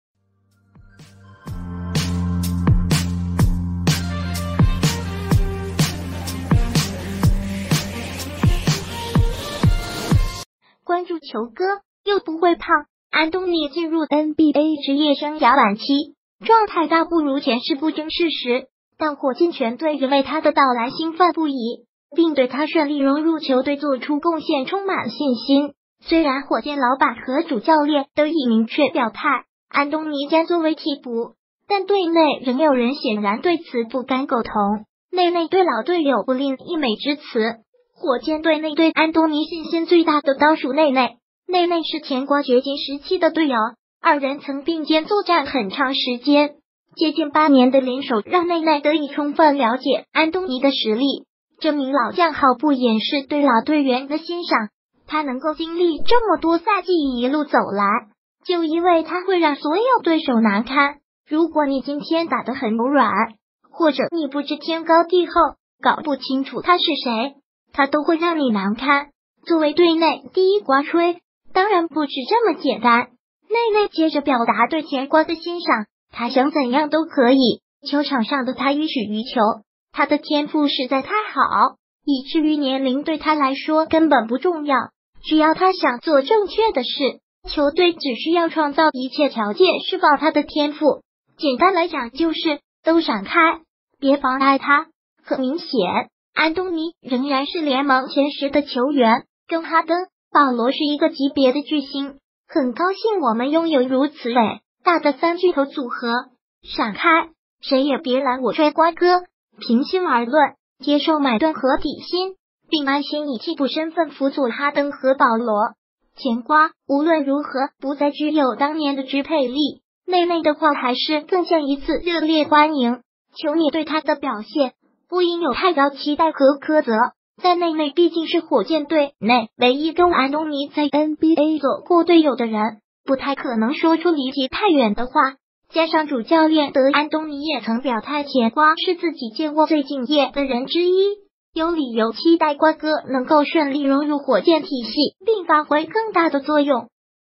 火箭队内第一瓜吹坐实 全联盟前10火箭球员至少3席？
关注球哥，又不会胖~┓( ??` )┏
            安东尼进入NBA职业生涯晚期，状态大不如前是不争事实，但火箭全队仍为他的到来兴奋不已，并对他顺利融入球队做出贡献充满信心。虽然火箭老板和主教练都已明确表态安东尼将作为替补，但队内仍有人显然对此不敢苟同。

            内内对老队友不吝溢美之词
            火箭队内对安东尼信心最大的当属内内。内内是甜瓜掘金时期的队友，二人曾并肩作战很长时间。接近8年的联手让内内得以充分了解安东尼的实力。这名老将毫不掩饰对老队员的欣赏：“他能够经历这么多赛季一路走来，就因为他会让所有对手难堪。如果你今天打得很软，或者你不知天高地厚，搞不清楚他是谁，他都会让你难堪。”
            作为队内第一瓜吹，当然不止这么简单。内内接着表达对甜瓜的欣赏：“他想怎样都可以，球场上的他予取予求。他的天赋实在太好，以至于年龄对他来说根本不重要。只要他想做正确的事，球队只需要创造一切条件释放他的天赋，简单来讲就是：都闪开，别妨碍他